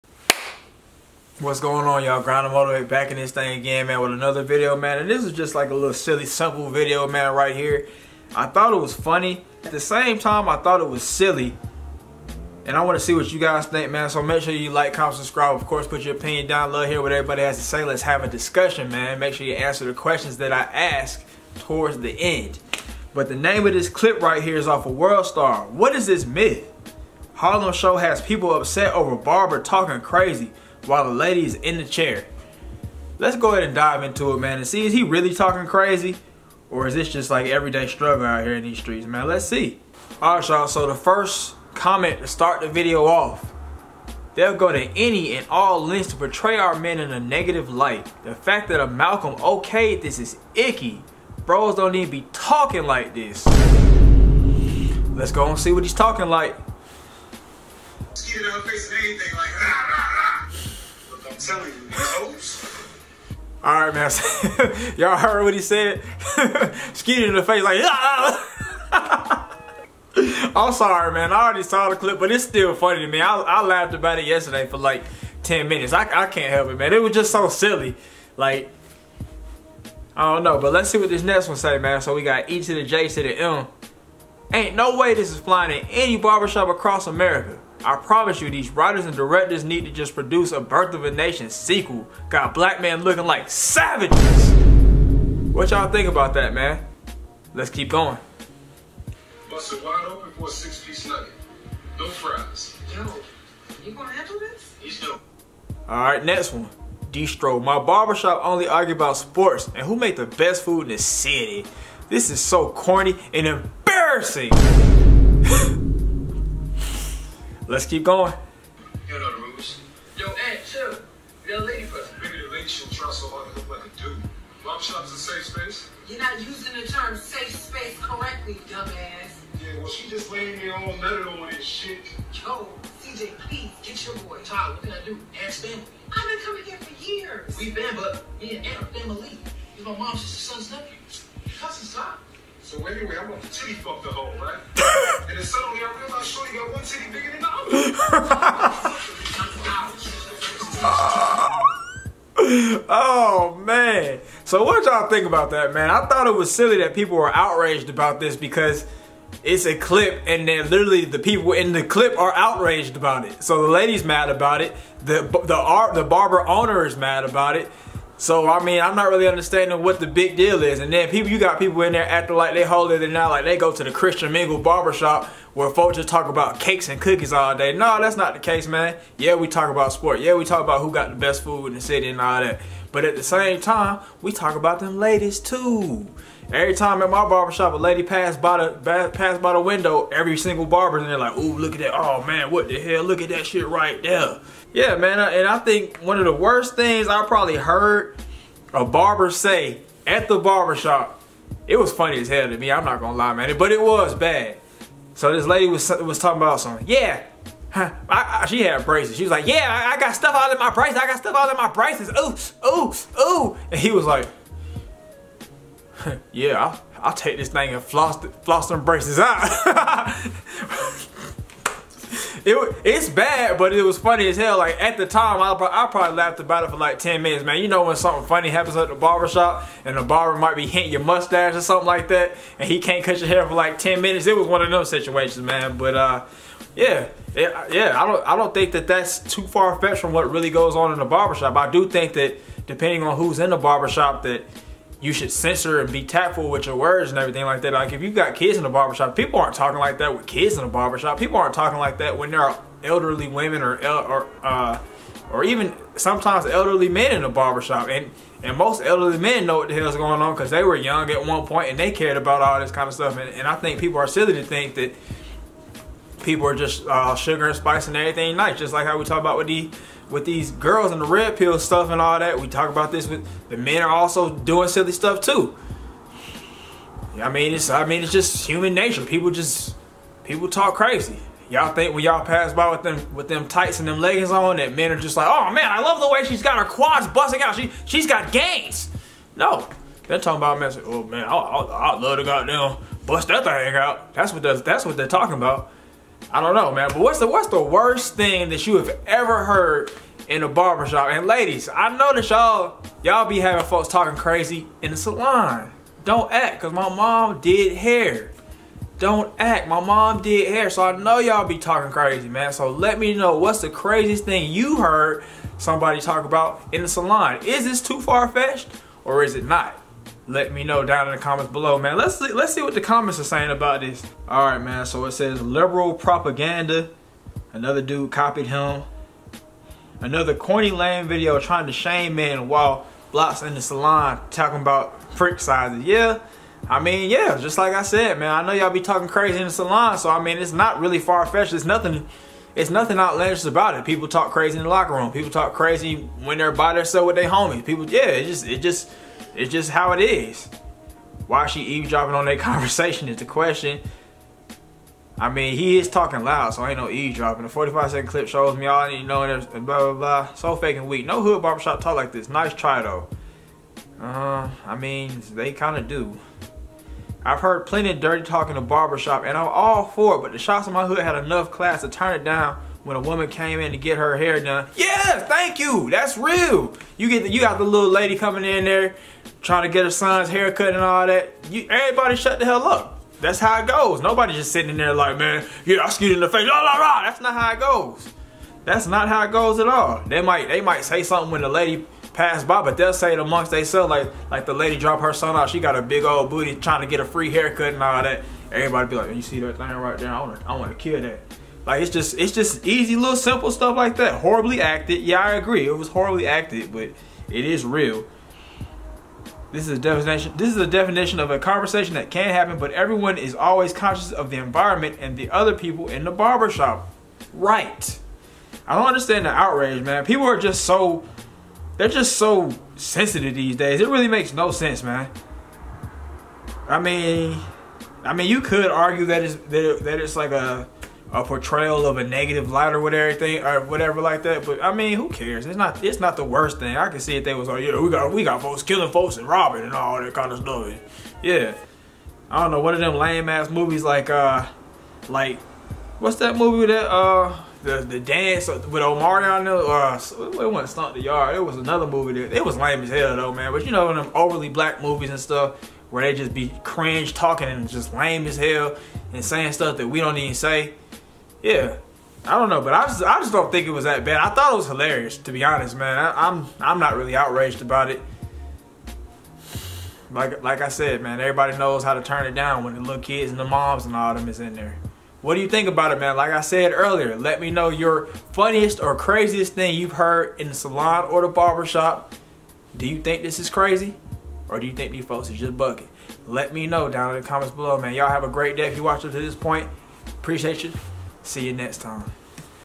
People are outraged by barbershop talk on Harlem tv show!? Are they justified or sensitive?
Go follow my I.G @kokidd45 @grindandmotivate45